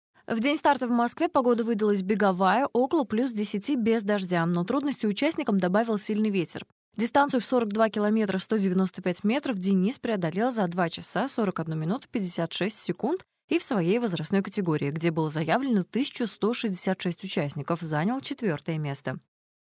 Московский марафон
Московский марафон в восьмой раз собрал любителей бега и элиту российской лёгкой атлетики -  проверить свои силы на дистанциях в 42 и 10 километров. На старт вышли свыше 30 тысяч участников, регион 13 на соревнованиях представляли 4 спортсмена. Среди них двукратный призер Олимпийских игр Денис Нижегородов.